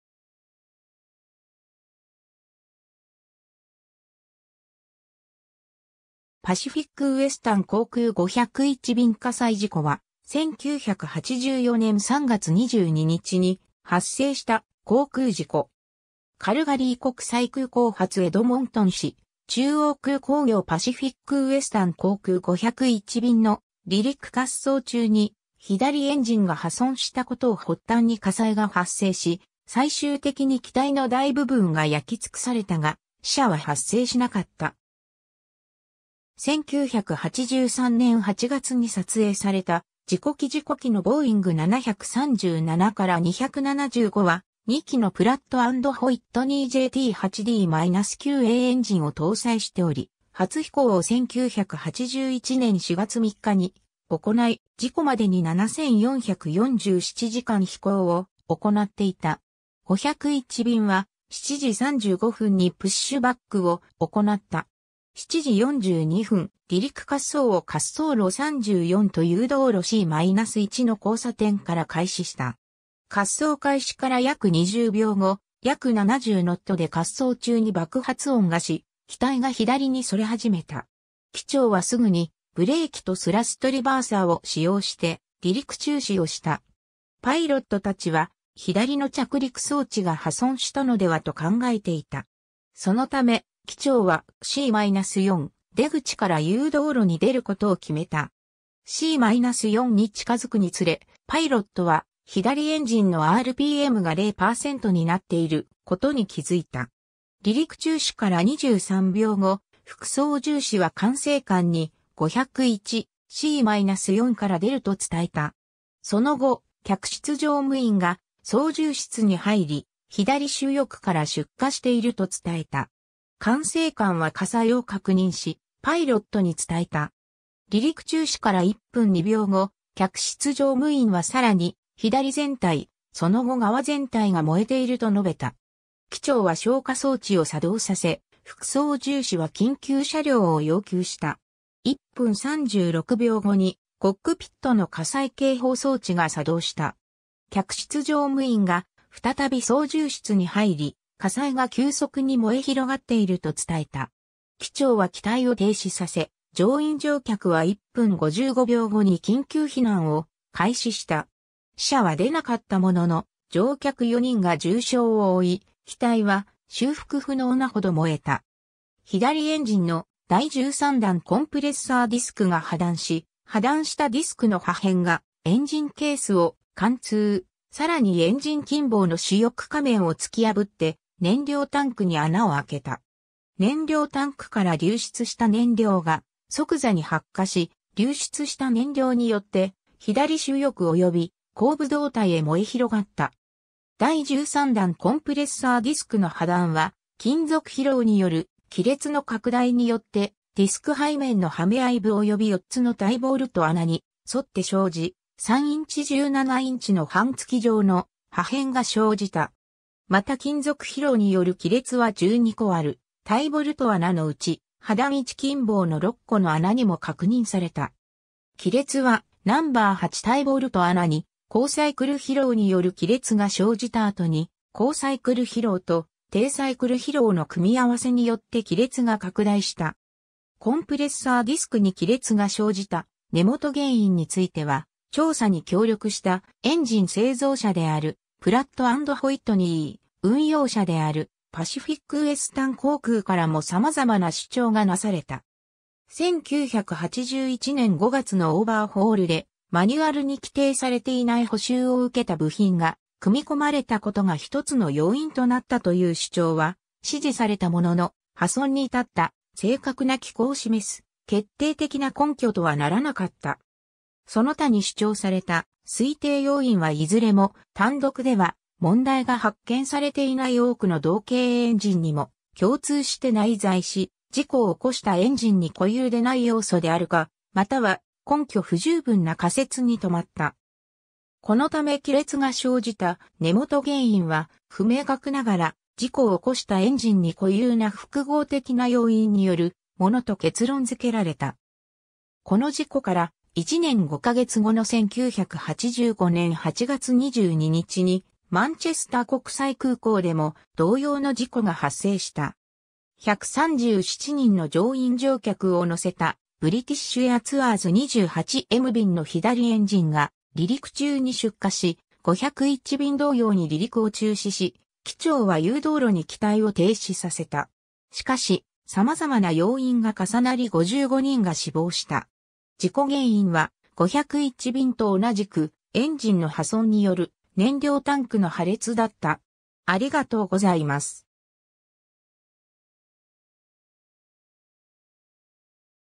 パシフィック・ウエスタン航空501便火災事故
パシフィック・ウエスタン航空501便火災事故は、1984年3月22日に発生した航空事故。
カルガリー国際空港発エドモントン市中央空港行パシフィック・ウエスタン航空501便（ボーイング737-200）の離陸滑走中に左エンジンが破損したことを発端に火災が発生し、最終的に機体の大部分が焼きつくされたが、死者は発生しなかった。
 1983年8月に撮影された事故機 事故機のボーイング737-275（C-GQPW）は、2基のプラット・アンド・ホイットニーJT8D-9Aエンジンを搭載しており、初飛行を1981年4月3日に行い、事故までに7,447時間飛行を行っていた。
 501便は、7時35分にプッシュバックを行った。
7時42分、離陸滑走を滑走路34と誘導路C-1の交差点から開始した。
滑走開始から約20秒後、約70ノットで滑走中に爆発音がし、機体が左に逸れ始めた。
機長はすぐにブレーキとスラストリバーサーを使用して離陸中止をした。
パイロット達は左の着陸装置が破損したのではと考えていた。
そのため、機長はC-4出口から誘導路に出ることを決めた。
C-4に近づくにつれ、パイロットは、左エンジンのrpmが0％になっていることに気付いた。
 離陸中止から23秒後、副操縦士は管制官に、「501、C-4から出る」と伝えた。
その後、客室乗務員が操縦室に入り、左主翼から出火していると伝えた。
管制官は火災を確認し、パイロットに伝えた。
離陸中止から1分2秒後、客室乗務員はさらに、「左全体、その後側全体が燃えている」と述べた。
機長は消火装置を作動させ、副操縦士は緊急車両を要求した。
 1分36秒後に、コックピットの火災警報装置が作動した。
客室乗務員が再び操縦室に入り、火災が急速に燃え広がっていると伝えた。
機長は機体を停止させ、乗員乗客は1分55秒後に緊急避難を開始した。
 死者は出なかったものの、乗客4人が重傷を負い、機体は修復不能なほど燃えた。
 左エンジンの第13段コンプレッサーディスクが破断し、破断したディスクの破片がエンジンケースを貫通、さらにエンジン近傍の主翼下面を突き破って燃料タンクに穴をあけた。
燃料タンクから流出した燃料が即座に発火し、流出した燃料によって左主翼および後部胴体へ燃え広がった。
 第13段コンプレッサーディスクの破断は金属疲労による亀裂の拡大によってディスク背面のはめあい部および4つのタイボルト穴に沿って生じ、3インチ×17インチの半月状の破片が生じた。
また金属疲労による亀裂は12個あるタイボルト穴のうち、破断位置近傍の6個の穴にも確認された。
亀裂は、No.8タイボルト穴に高サイクル疲労による亀裂が生じた後に高サイクル疲労と低サイクル疲労の組み合わせによって亀裂が拡大した。
 コンプレッサーディスクに亀裂が生じた根本原因については、調査に協力したエンジン製造者であるプラット・アンド・ホイットニー、運用者であるパシフィック・ウエスタン航空からも様々な主張がなされた。
1981年5月のオーバーホールでマニュアルに規定されていない補修を受けた部品が組み込まれたことが一つの要因となったという主張は支持されたものの、破損に至った正確な機構示す決定的な根拠とはならなかった。
その他に主張された推定要因はいずれも、単独では問題が発見されていない多くの同型エンジンにも共通して内在し、事故を起こしたエンジンに固有でない要素であるか、または根拠不十分な仮説に留まった。
このため亀裂が生じた根本原因は不明確ながら、事故を起こしたエンジンに固有な複合的な要因によるものと結論付けられた。
 この事故から、1年5ヶ月後の1985年8月22日に、マンチェスター国際空港でも同様の事故が発生した。
137人の乗員乗客をのせた、ブリティッシュ・エアツアーズ28M便（ボーイング737-236）の左エンジンが離陸中に出火し、501便同様に離陸を中止し、機長は誘導路に機体を停止させた。
しかし、様々な要因が重なり55人が死亡した。
事故原因は、501便と同じく、エンジンの破損による燃料タンクの破裂だった。